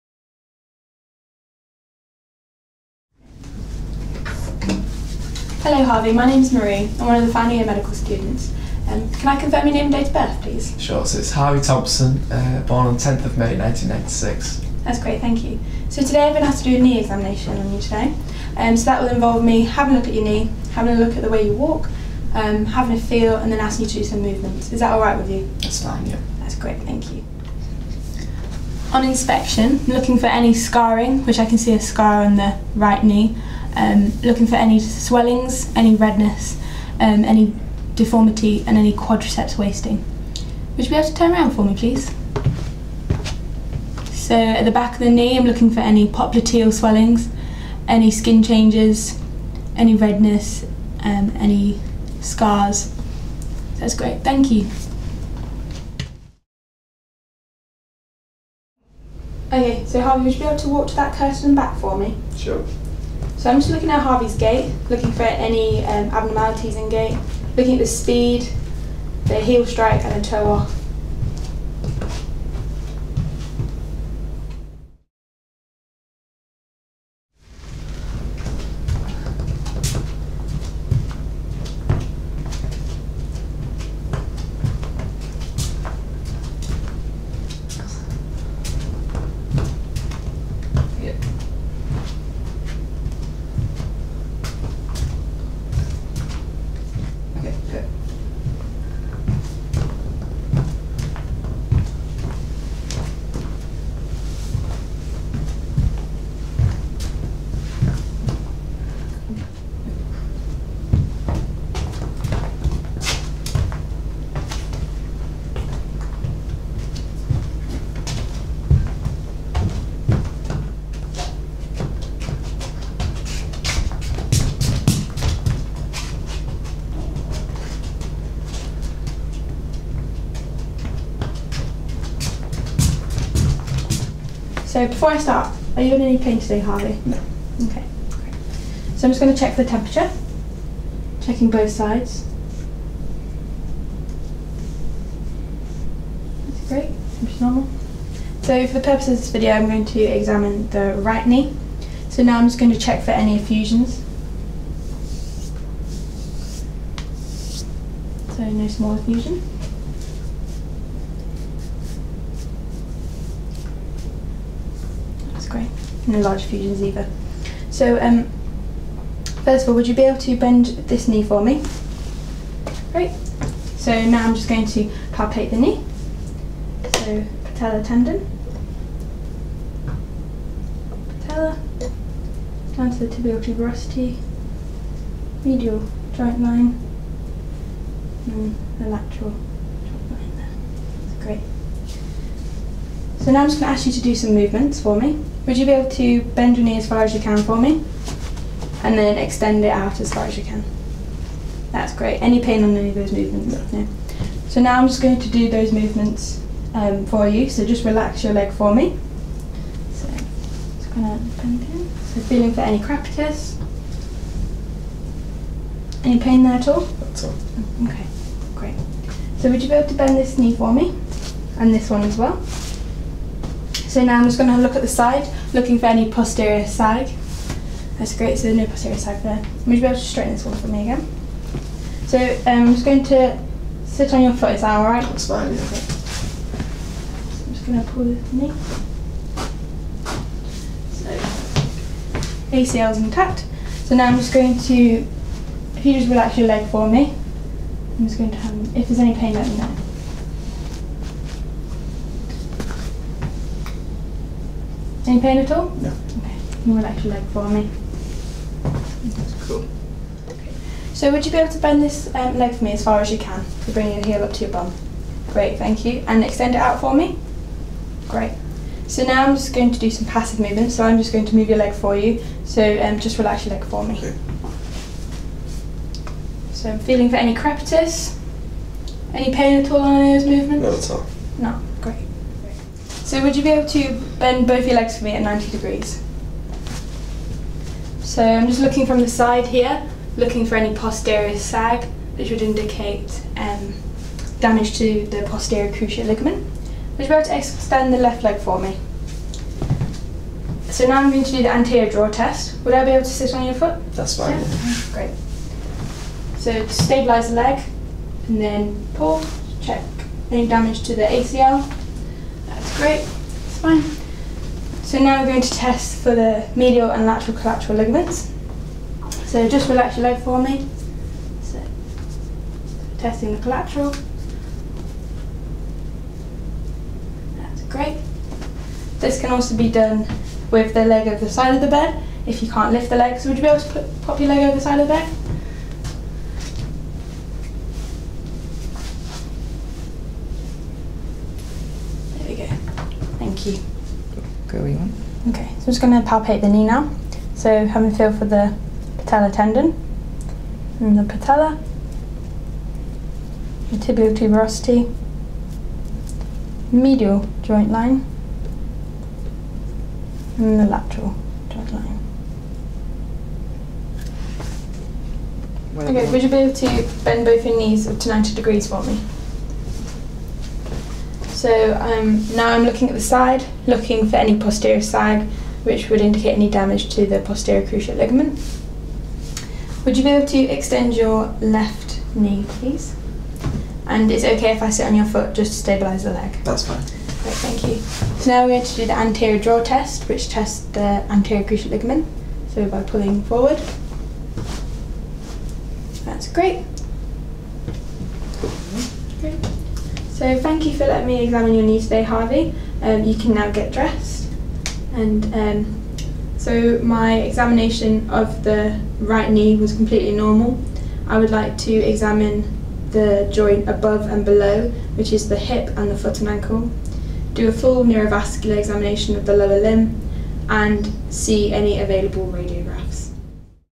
Knee Examination
Demonstration of a knee examination, by Leicester Medical School senior students. Includes examples of abnormal gait.